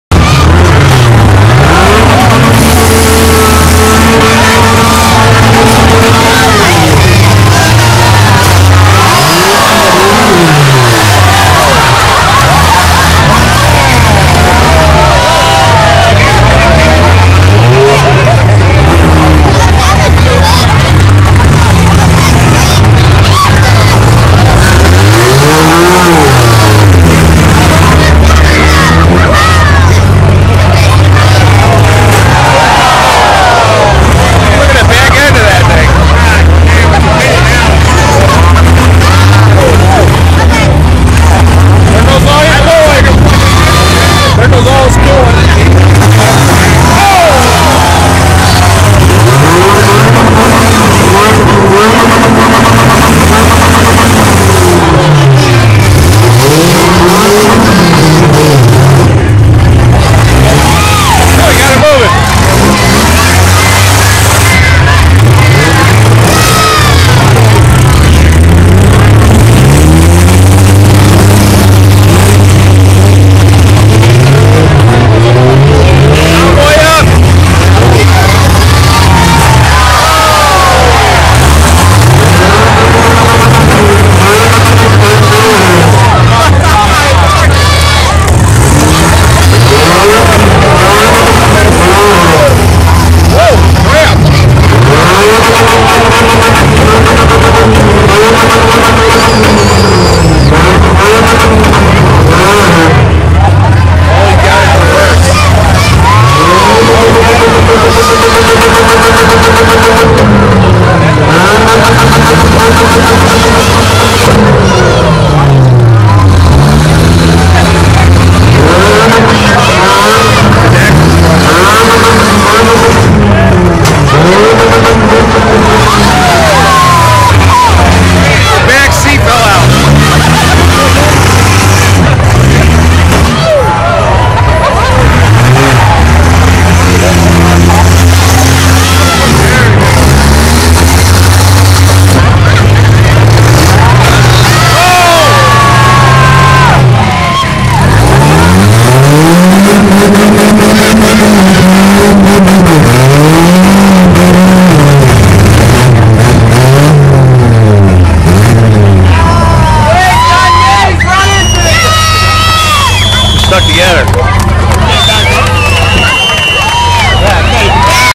The third set of the night, sub compact rematch
After the win of last race, round two, just because there was still too many cars left drivable after the first